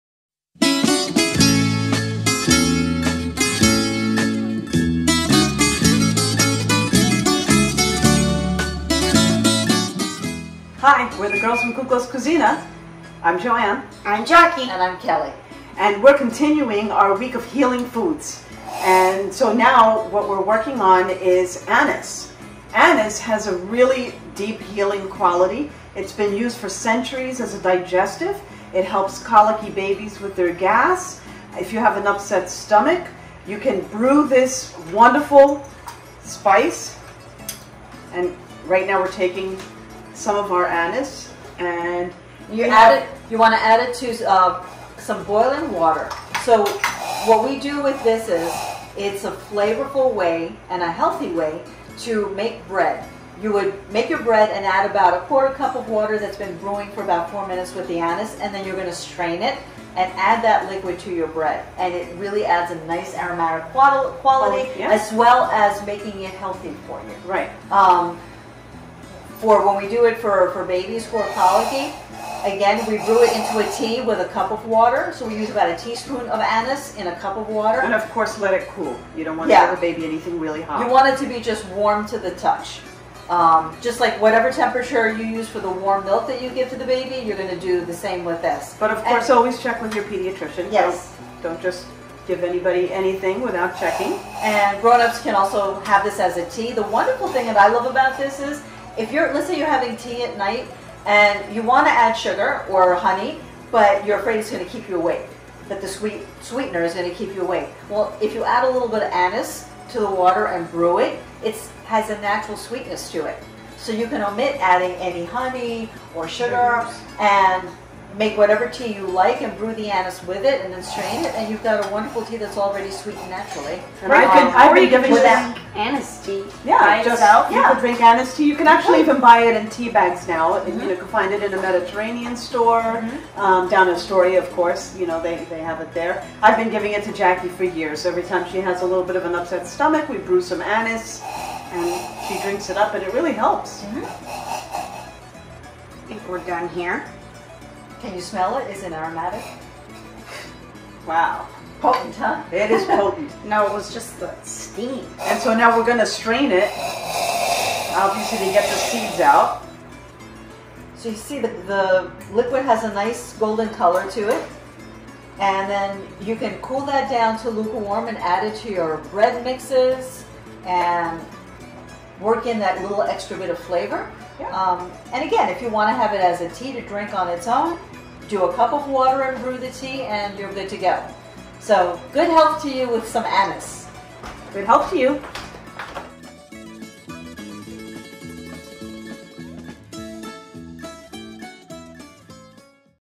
E146 Healing Greek Spice~Anise Seed
Anise seed is a healing spice with antifungal, antibacterial, and anti-inflammatory properties that aid digestion. We brewed the seeds into an aromatic, naturally-sweet tea to drink or to flavor bread, deliciously adding a healthy element to daily life. Growing up, as well as today, this remedy always worked for us.

Check with your doctor regarding interactions with medicines or any health concerns.